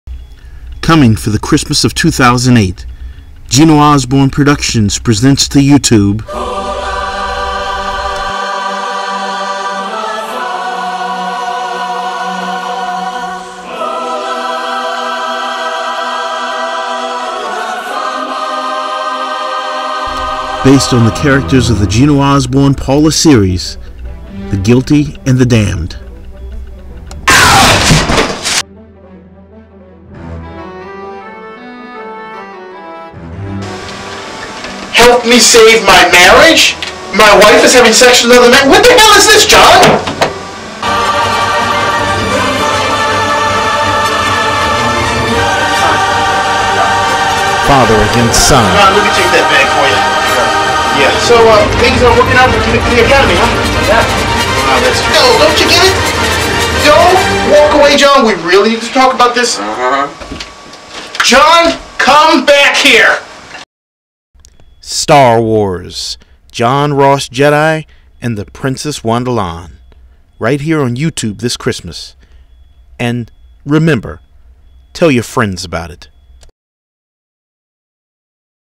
John Ross Jedi movie preview
Sign in to YouTube now!
Sign in with your Google Account!
Ok you tube fans. This is my long awaited short vers...
Ok you tube fans. This is my long awaited short version of my star wars fan film; John Ross Jedi which will be released this christmas. Enjoy, and tell your friends about it...